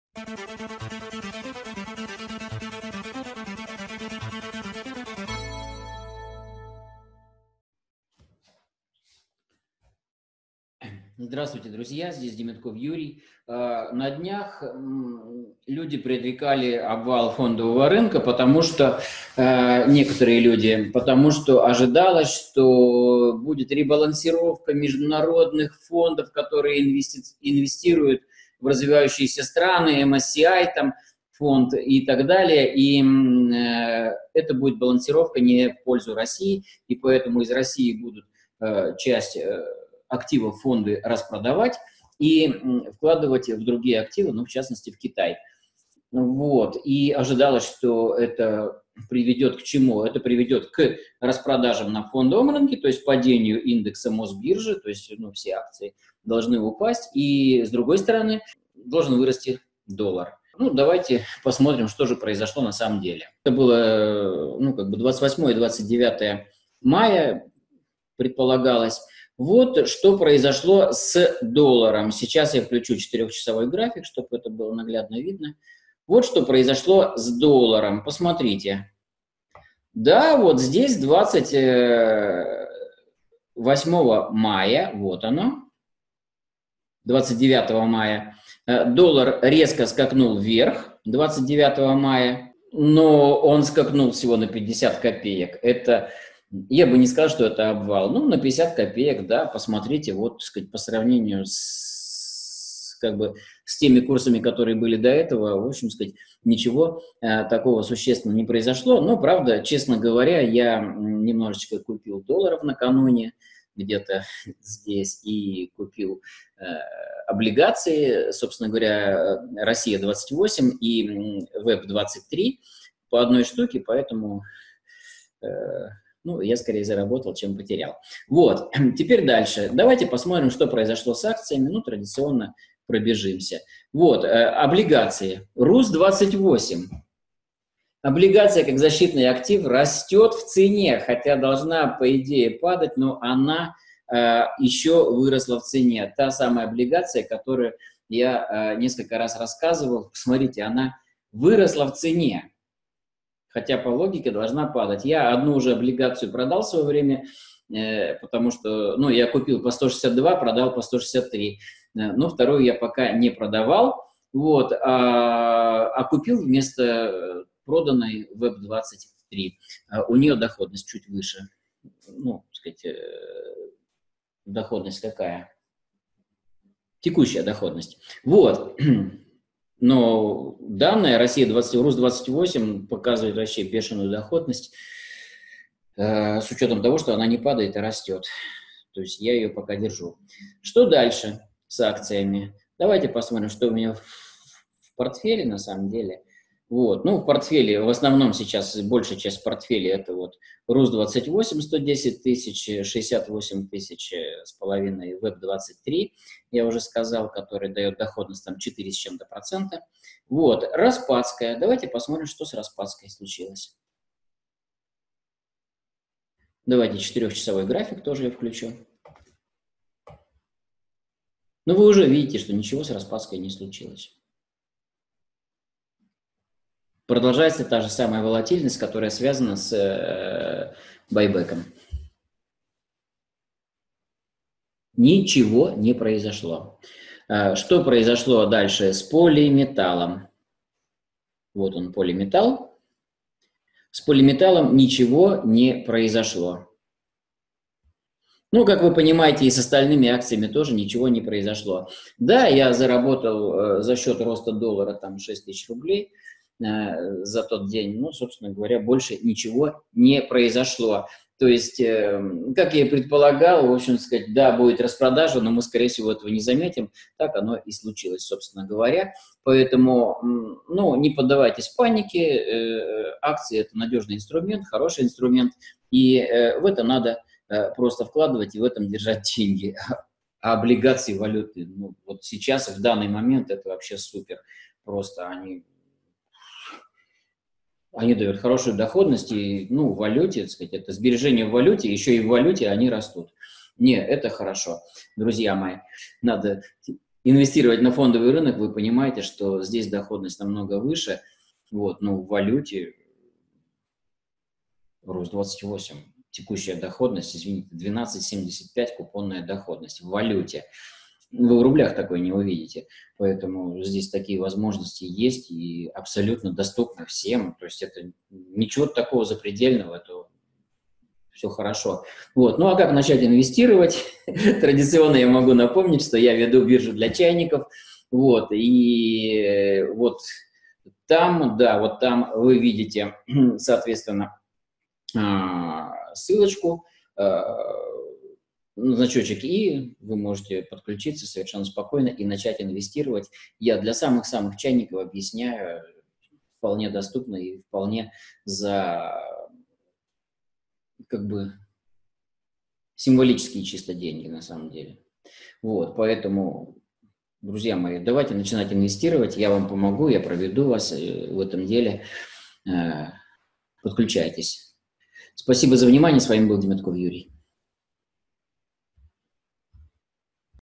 Обвал рынка: никто не заметил.
Некоторые трейдеры предрекали значительное падение фондового рынка России 28-29 мая в связи с распродажами российских активов. Что произошло в реальности?